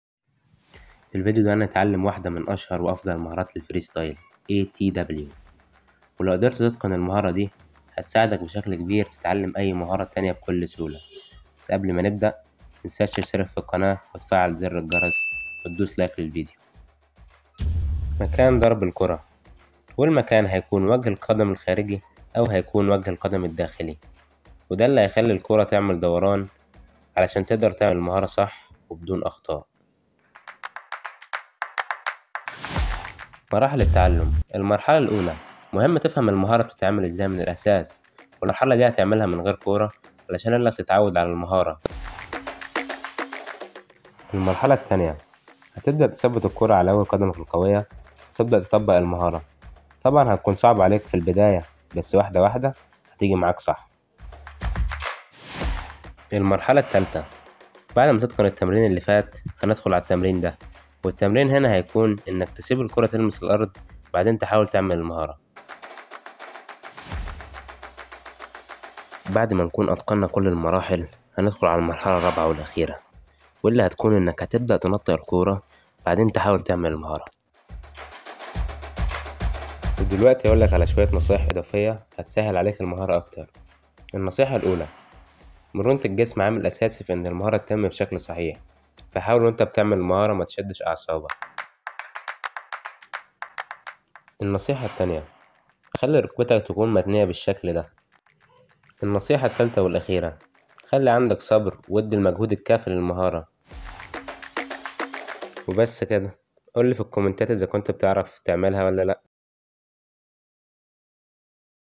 تعلم كيف تلف قدمك حول الكرة بأسهل طريقه🔥🤯
ازاي الف رجلي حوالين الكرة بأسهل طريقه

لو الفيديو فادك بأي حاجة متنساش تدعم اخوك علوه❤️ب
1اشتراك. ✔️      2لايك. ✔️        مشاركه الفيديو✔️
واخيرا صلي على النبي ❤️❤️❤️